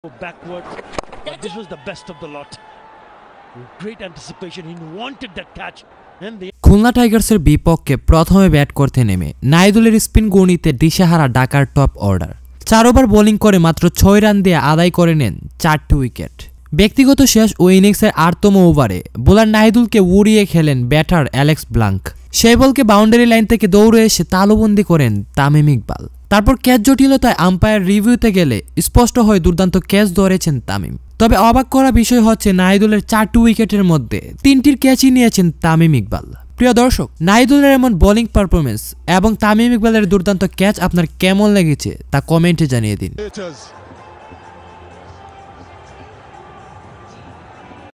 তামিমের দুর্দান্ত ক্যাচের পর রেকর্ড গড়লেন নাহিদুল! নাহিদুলে মুগ্ধ ক্রিকেটপ্রেমীরা
Welcome to @SportsDailyOfficial  Youtube channel.